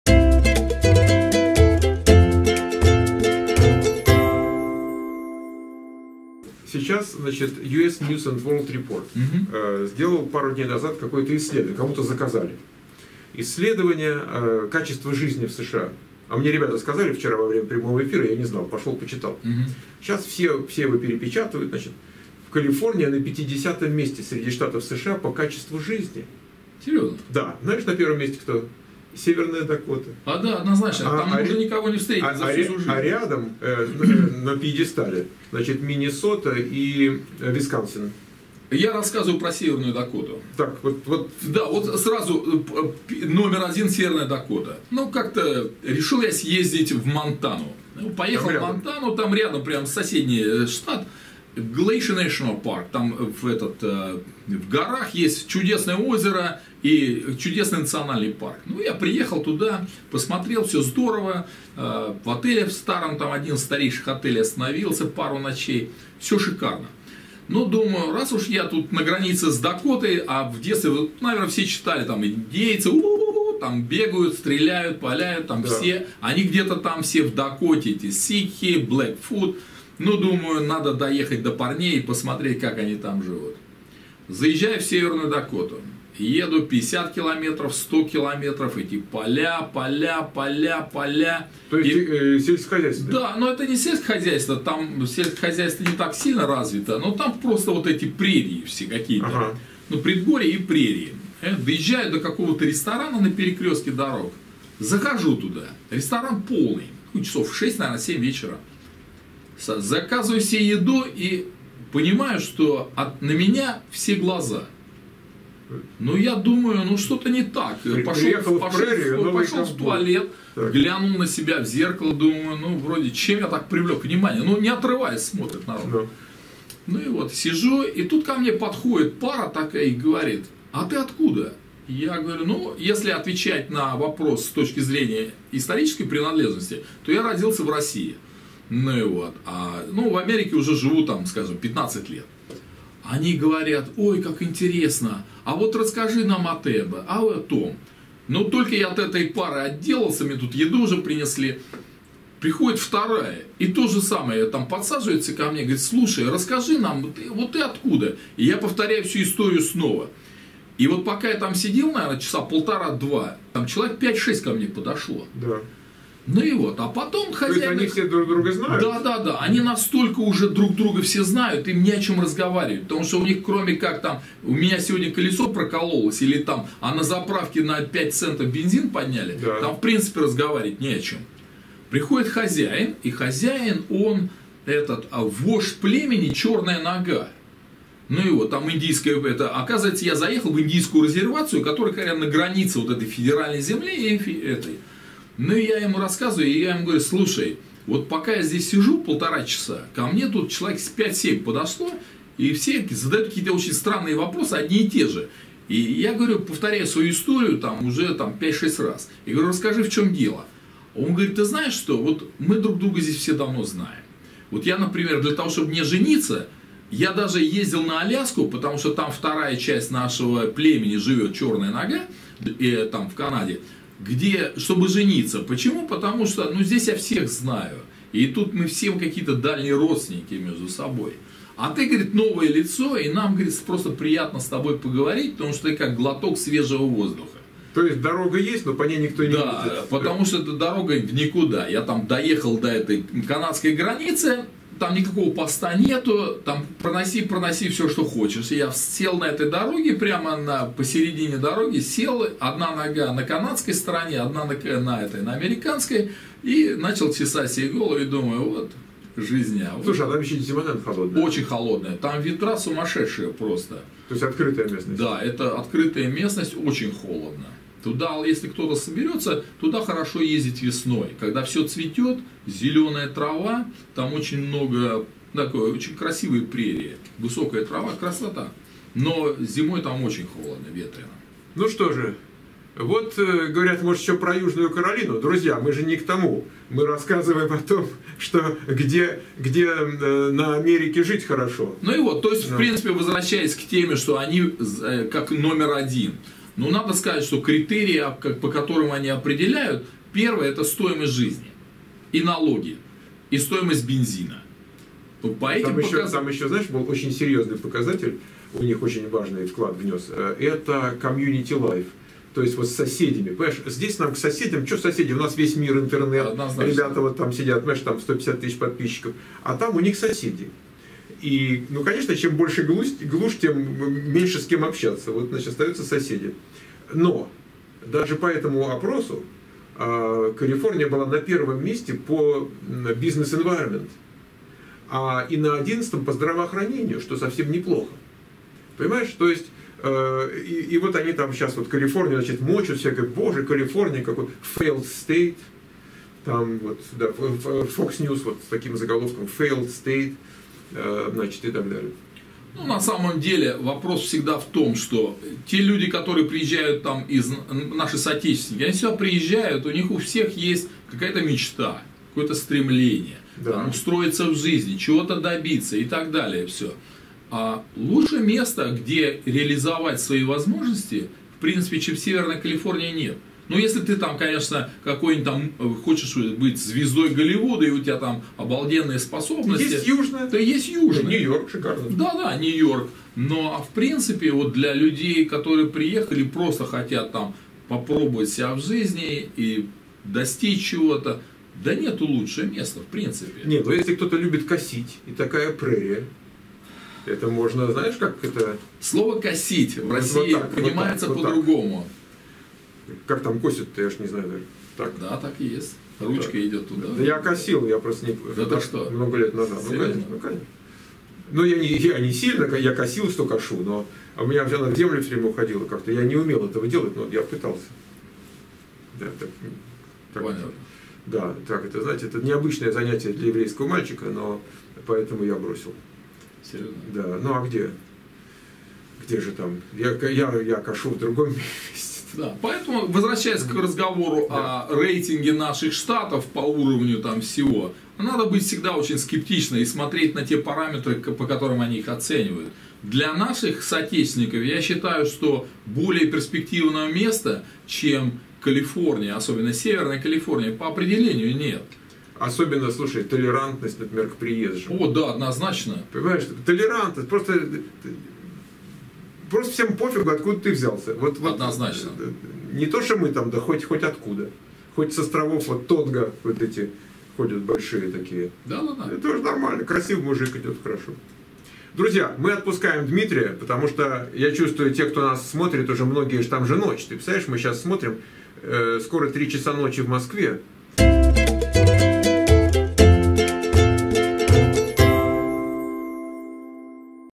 США 5125: Северная Калифорния или Северная Дакота - где ты, иммигрантская мечта?
Северная Калифорния или Северная Дакота - эксклюзивный репортаж от нашего корреспондента
Ссылки на канале SiliconValleyVoice /школа Михаил Портнов: